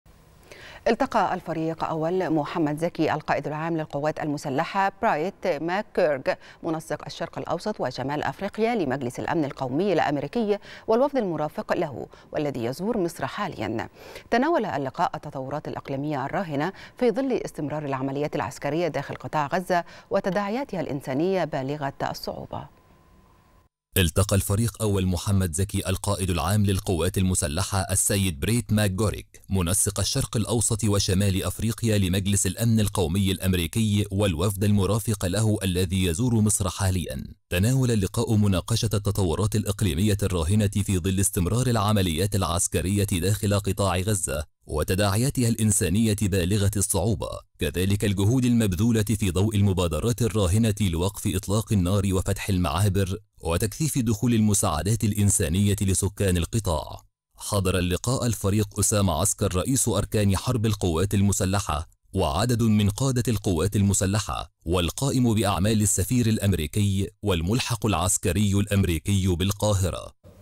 القائد العام للقوات المسلحة يلتقي منسق الشرق الأوسط وشمال أفريقيا لمجلس الأمن القومي الأمريكي
نشرة السادسة | الفريق أول محمد زكي القائد العام للقوات المسلحة يلتقي منسق الشرق الأوسط وشمال أفريقيا لمجلس الأمن القومي الأمريكي